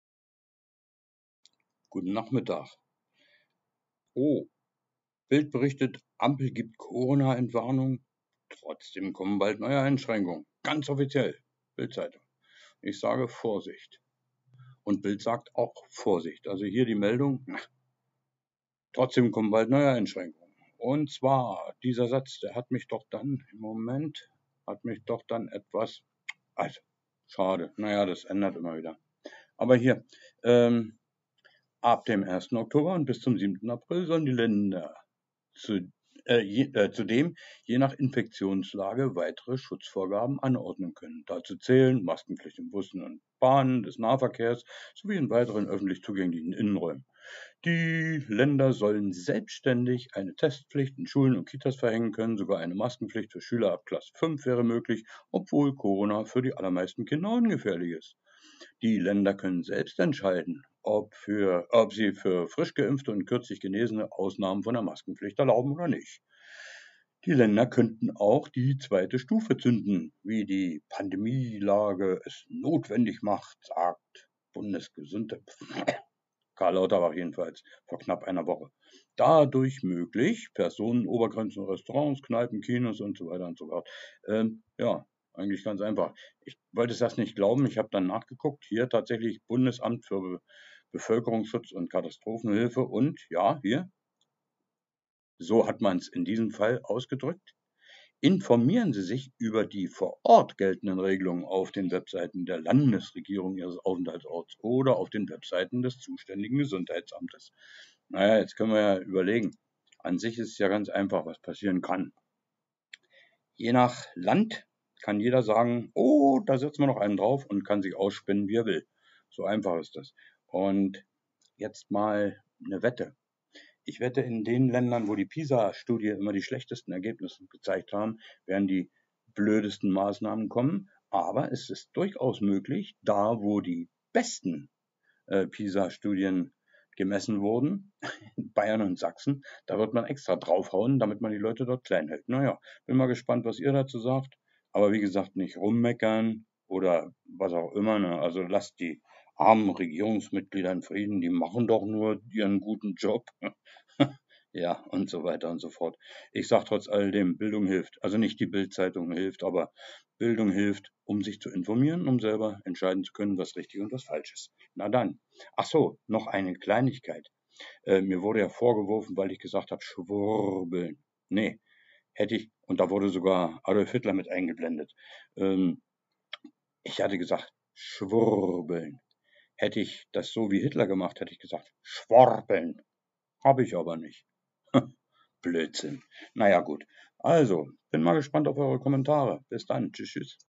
"Ampel gibt Corona Entwarnung", sagt die "BILD". Vorsicht!
+++ Meinungsfreiheit im MaPhy-Forum +++ Die Bundesländer können selbst entscheiden dank Infektionsschutzgesetz; das ist der Trick. Ist es Angst?

Werbung, also der Link zu meinem zweiten Buch "IQ ..."

Björn Köhler